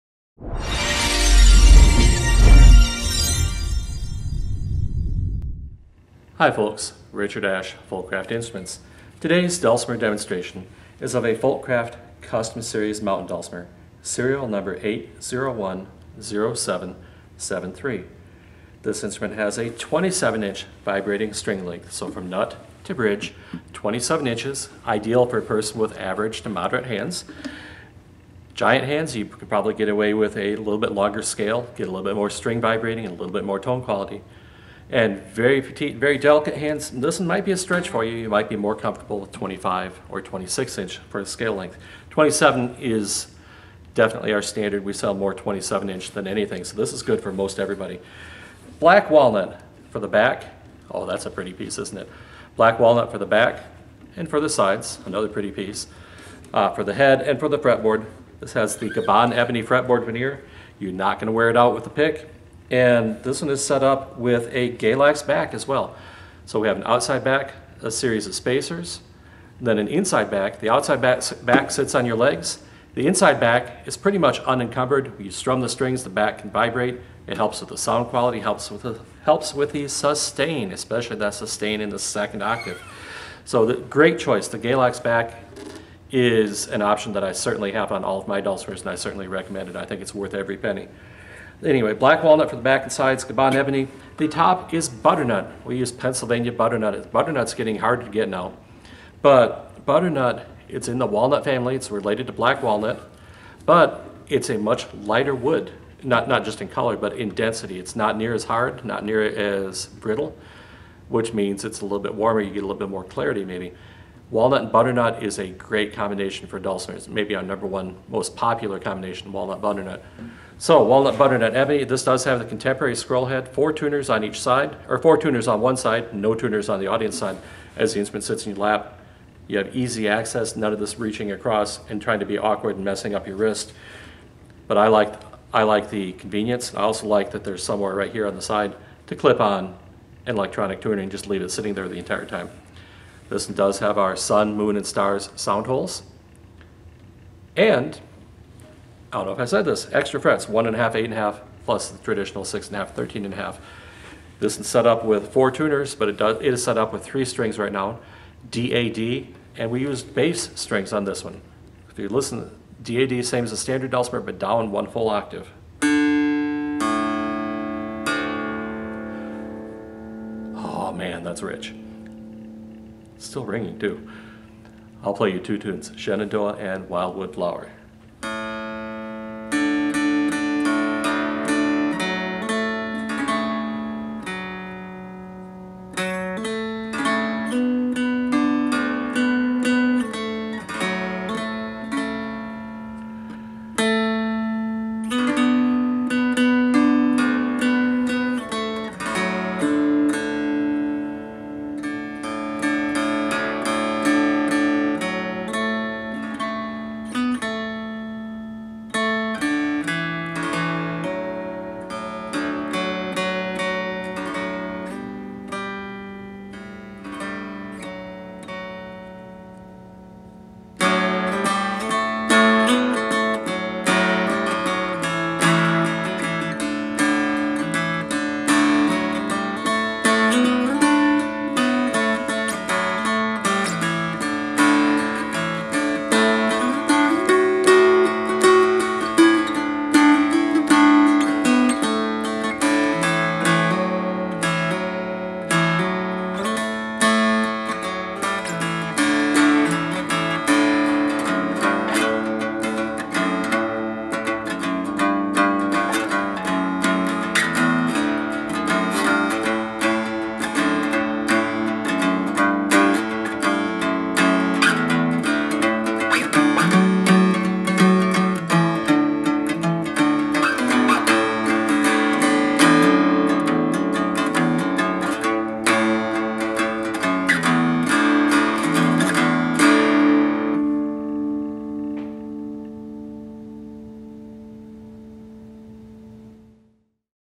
Folkcraft® Instruments Bass Dulcimer, Serial Number 8010773
Walnut body, butternut top. 3-string bass mountain dulcimer.